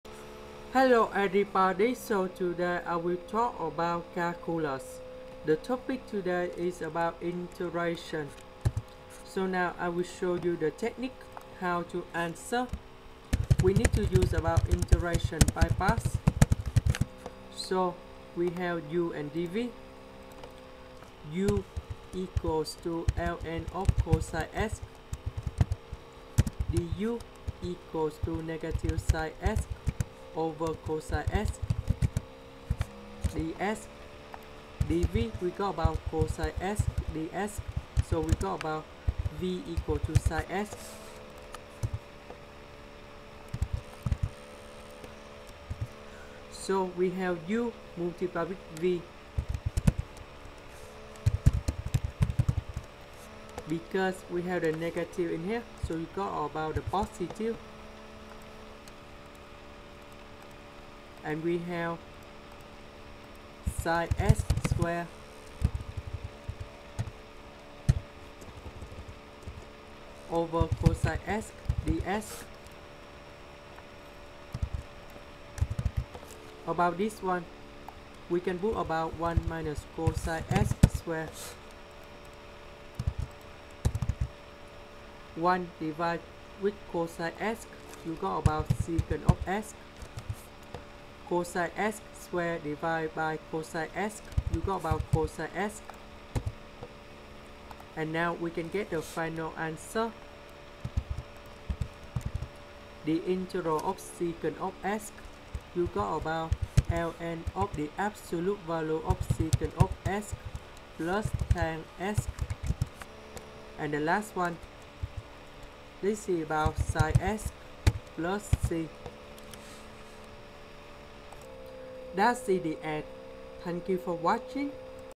Calculus Help: Integral of ∫ cosx ln⁡(cosx)dx - Integration by parts
Here is the technique to solve this integration and how to find